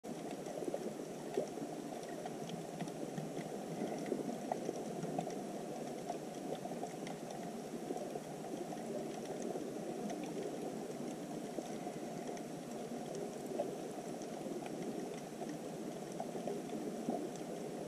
Hermit crab peeks out
Hard to tell, but this hermit crab was in a shell about the size of a regulation football!